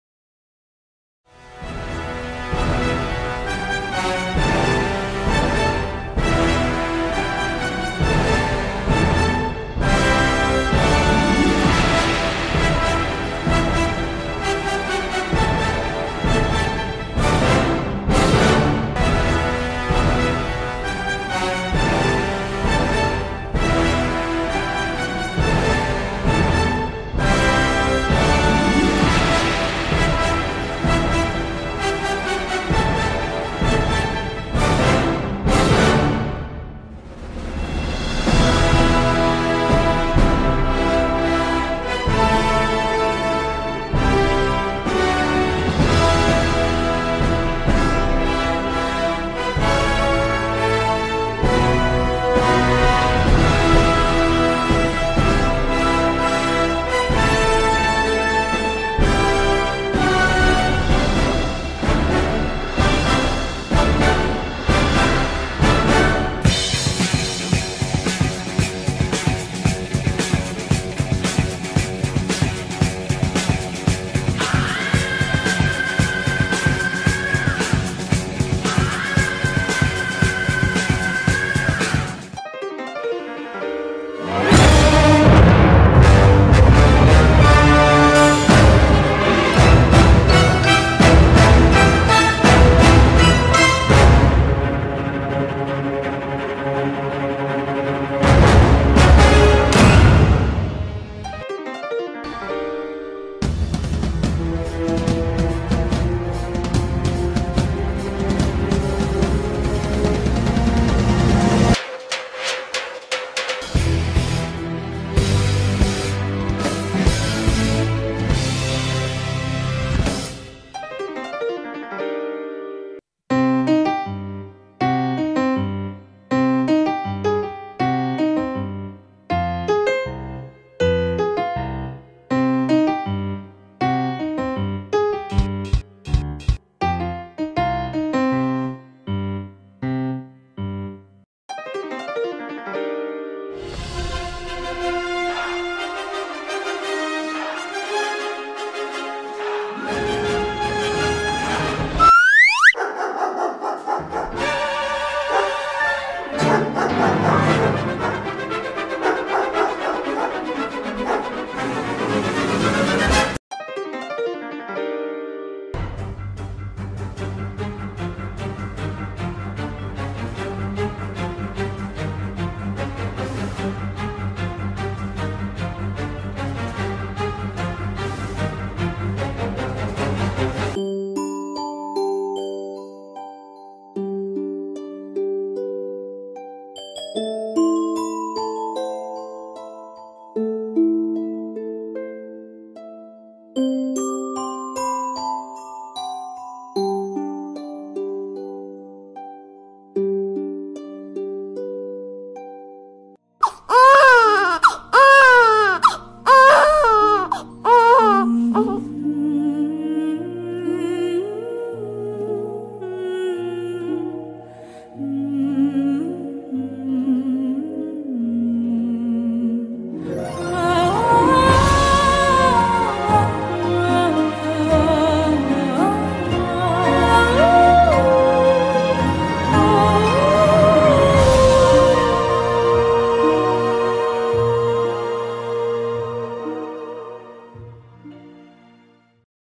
Event- 5 MIME - Title: Udbhava the beginning (Competition)
Title- Udbhava the beginning  (Competition)
Event- 5 MIME
Theme- Head of Avengers
About- As we all know according to Marvel the greatest heroes of the universe are Avengers and we are going to show some glimpse of Avengers. According to us the head of any superhero is a mother who has such incredible power that no superhero can come near to her abilities.
Participants- Krittika Garg, Japji Kaur, Aartiekka, Aditi Kapadia.
Class- 8A
School- The Millennium School Patiala.